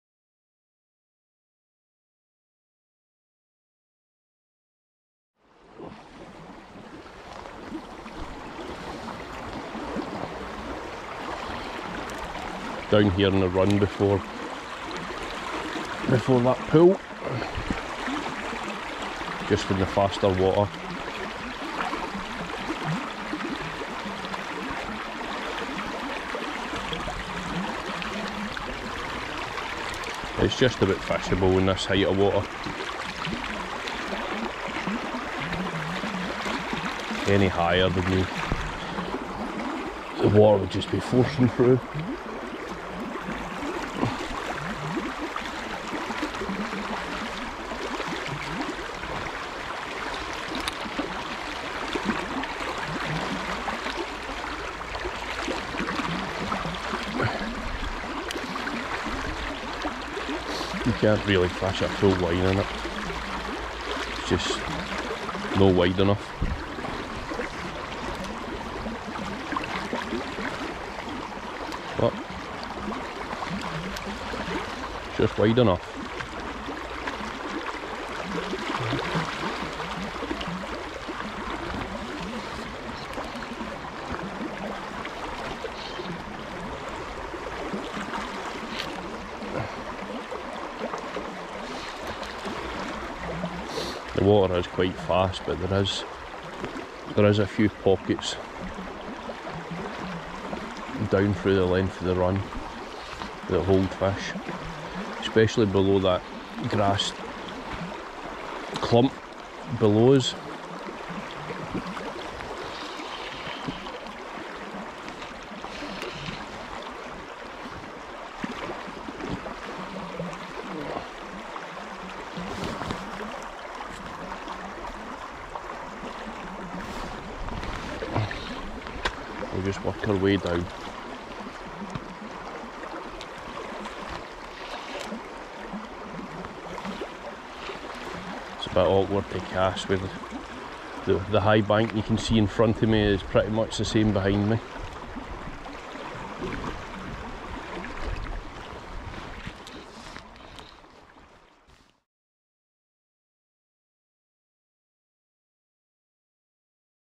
SALMON FISHING ON THE RIVER TWEED AT KAILZIE NOVEMBER 2020 PART 2
Fishing the island pool on Kailzie's upper beat on the river Tweed